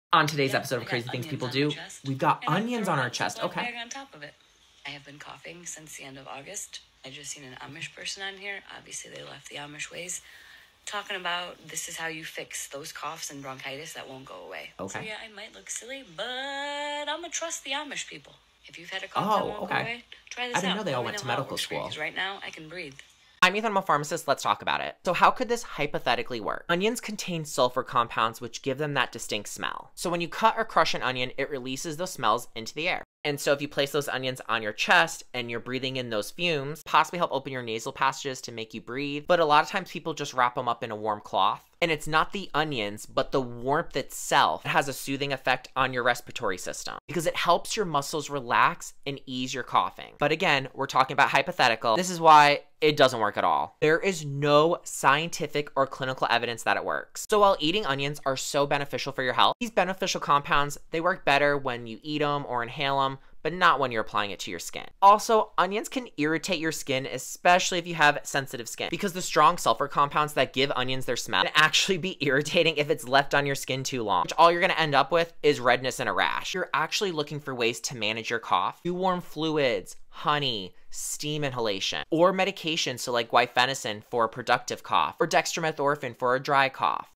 ONIONS CURE COUGHS?!? Pharmacist Debunks.
How could oniojs possibly help your cough but also lead to worse side effects.

‼️FOR MORE EDUCATIONAL CONTENT FROM DR. MELILLO‼️